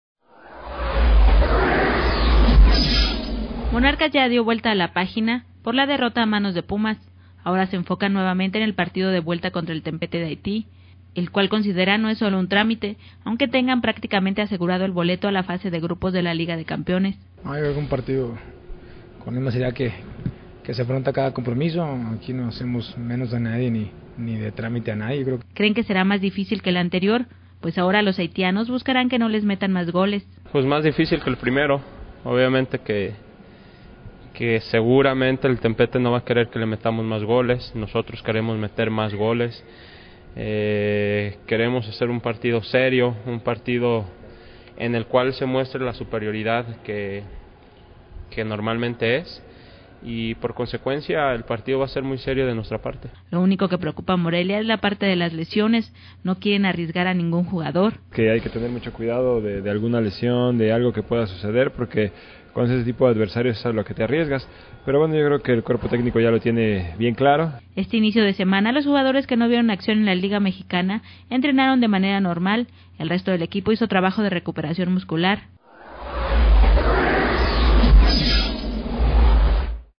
Monarcas Morelia Tempete No es un Tramite
Monarcas Morelia Tempete No es un Tramite dicen los jugadores del Equipo ya que evitaran recibir mas goles y con laa desventaja de no estar en casa, recibe a Morelia que ve las cosas tan enserio que jugara con un Equipo Alternativo